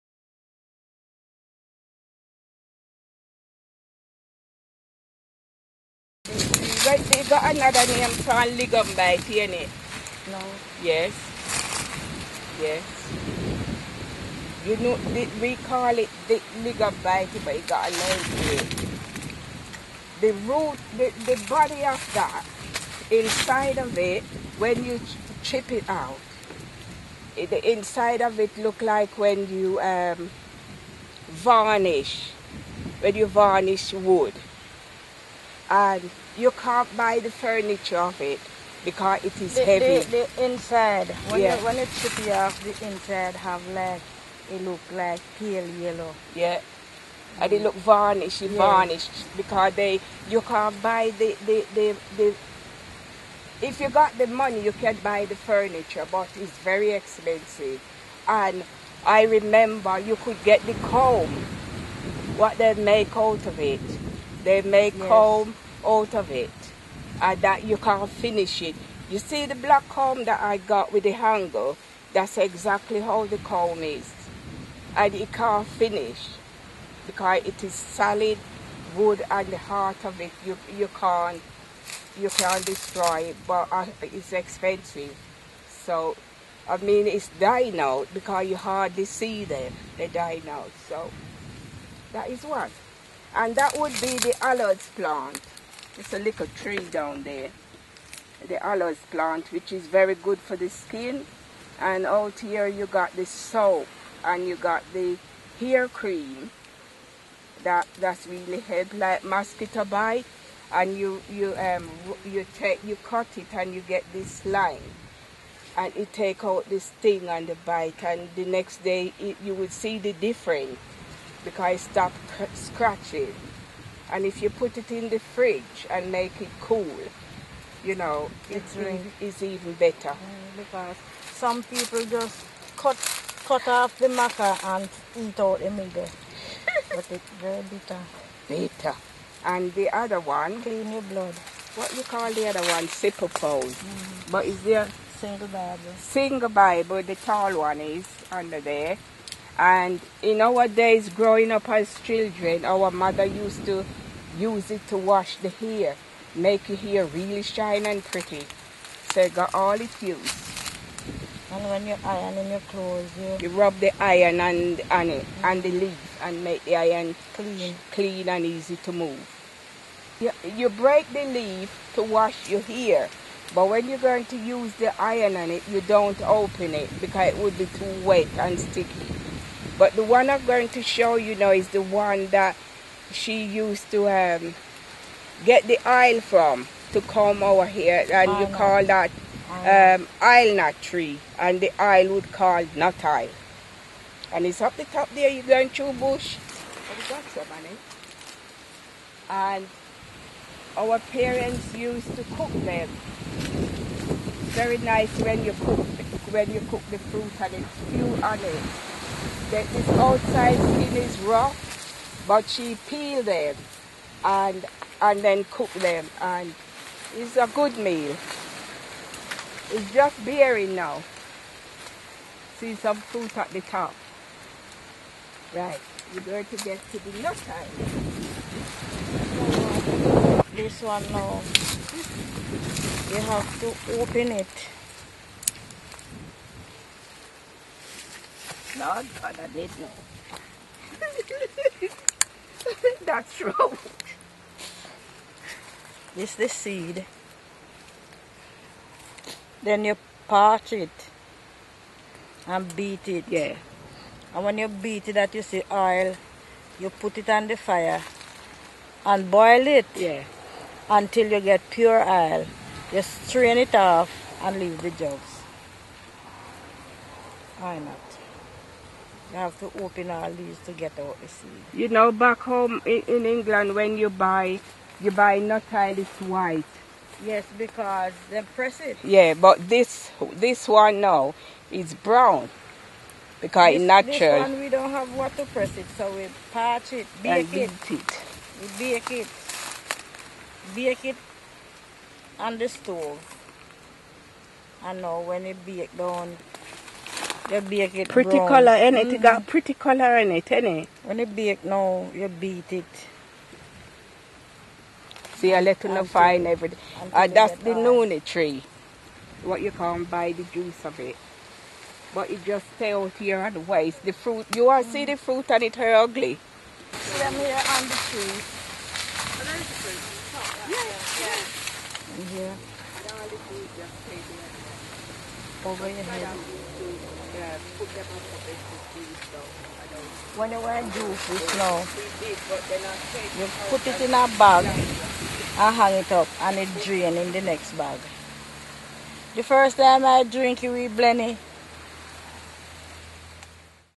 Origins of the Afro Comb: Eunice and Dorothy
Sisters Eunice and Dorothy talk about natural products that they used on their hair as children and the type of wood that was traditionally used to make hair combs. Fimed in St. Elizabeth JA.